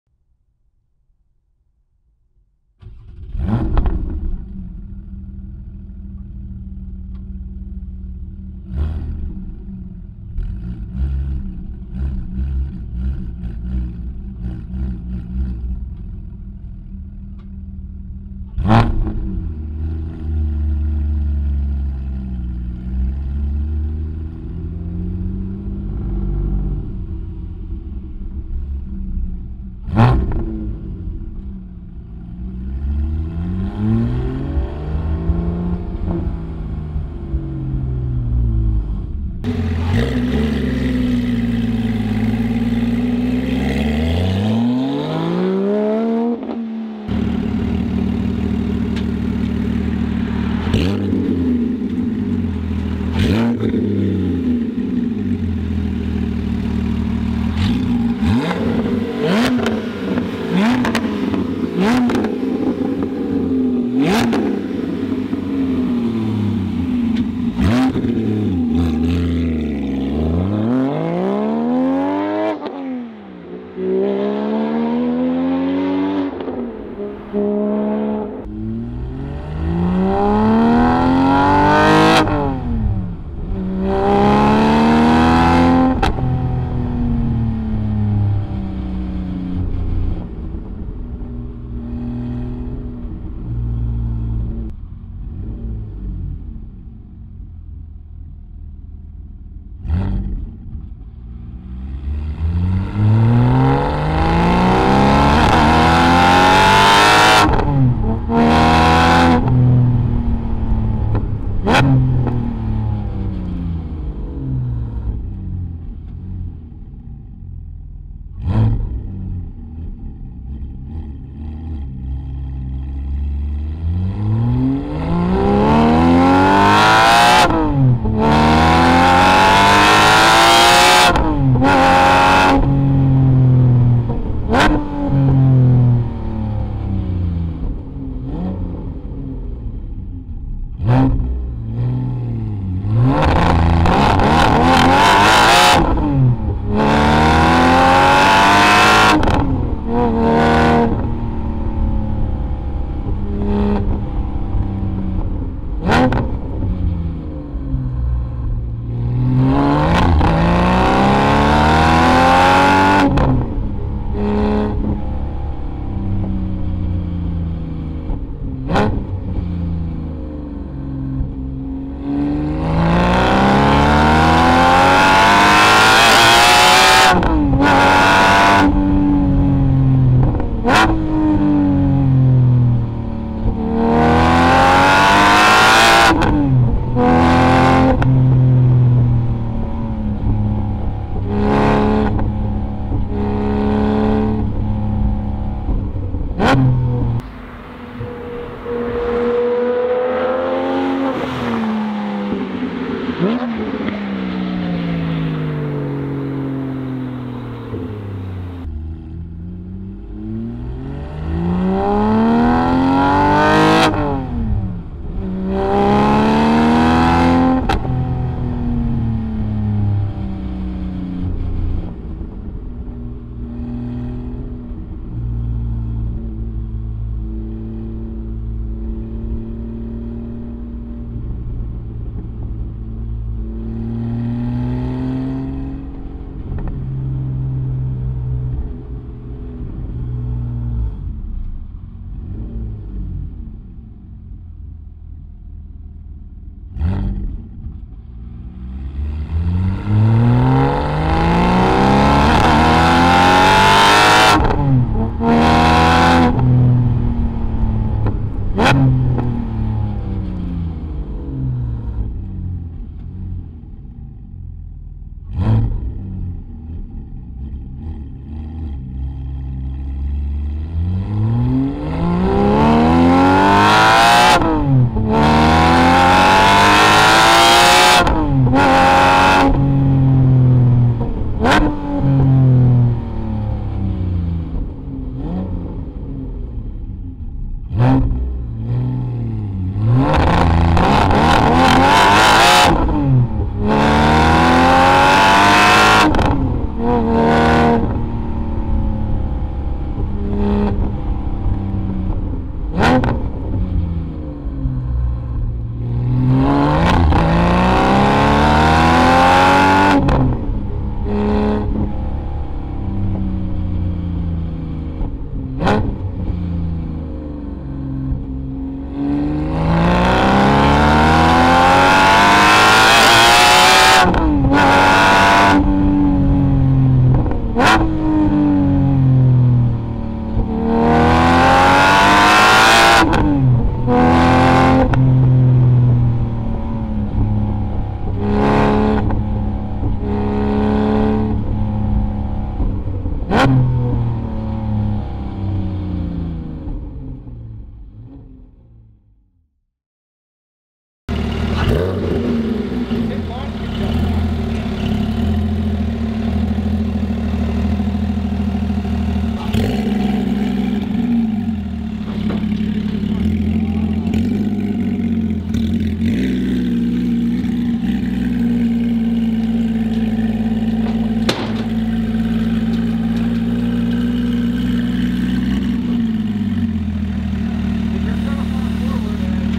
Porsche 718 GT4 / Spyder With Kline Innovation Inconel Full Exhaust
Just finished installing and recording the full exhaust of the GT4! 200 CEL cats and Inconel!

Main Camera
Main GoPro
Main Shotgun Mic
Main Lav Mic
Backup GoPro